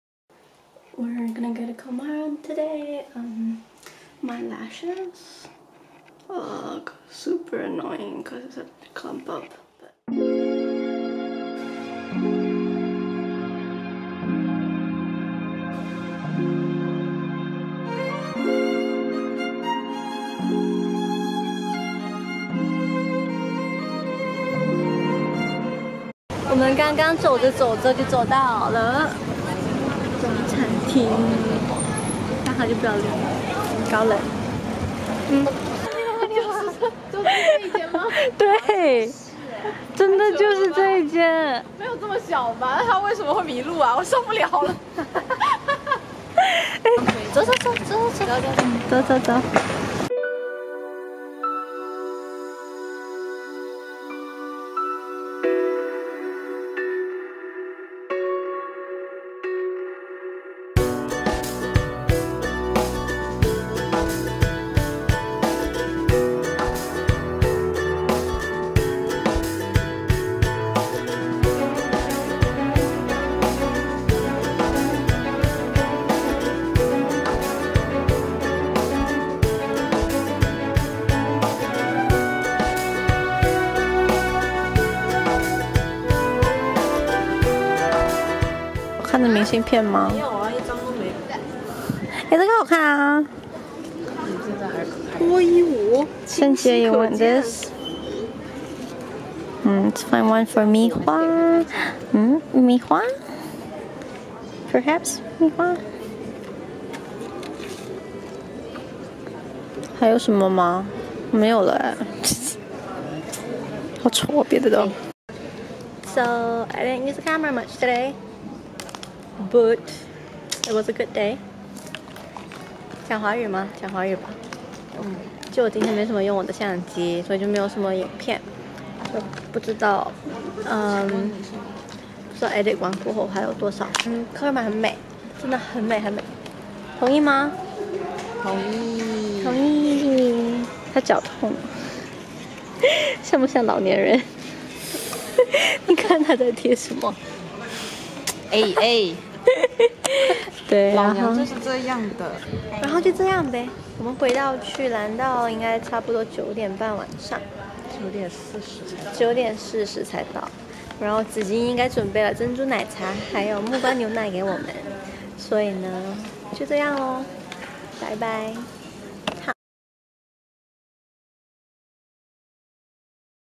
Colmar (CN / EN)
08/09/18 (Saturday)

Highlights:
1. Visited where Howl's Moving Castle was inspired
2. 中餐厅
3. Really really pretty architecture